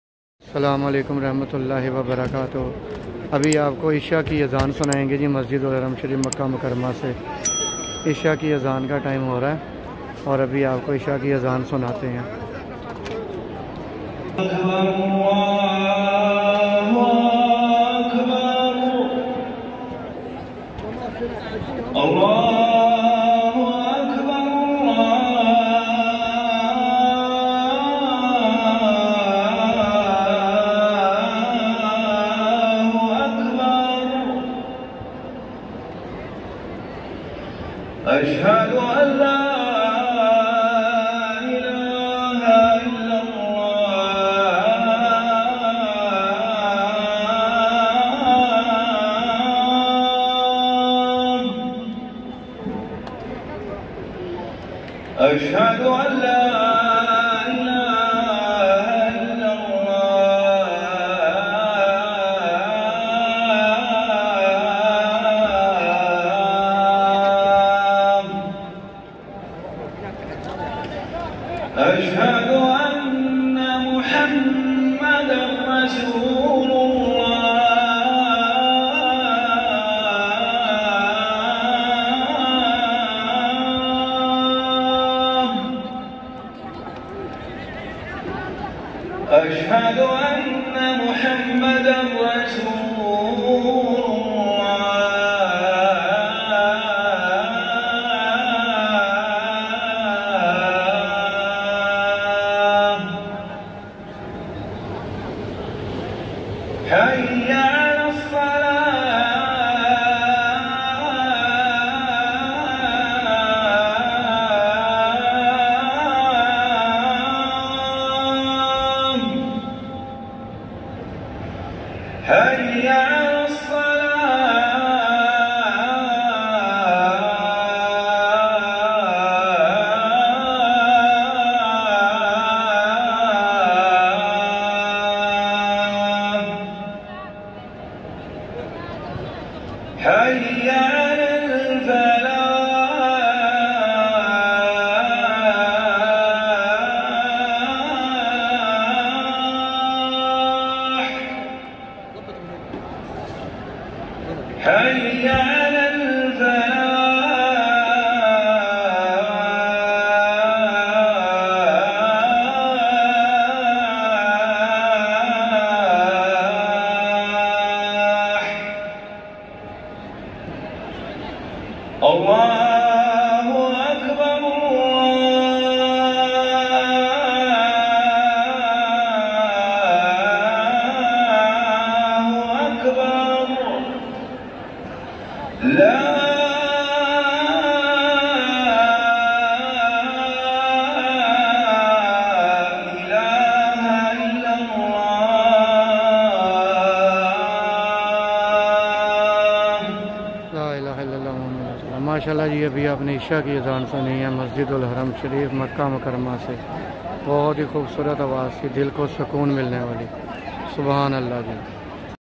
مسجد الحرام شریف مکہ مکرمہ سے عشاء کی ازان سنے
buttyfullviewsmasjid ul haram,